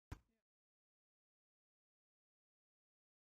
Vorhandwurf (2)
Das Mannschaftsspiel für Groß und Klein. Das Spiel mit der Wurfscheibe kann in vielerlei Varianten und mit viel Spaß gespielt werden, die Technik hierzu kann mithilfe des Beitrages ganz leicht erlernt werden.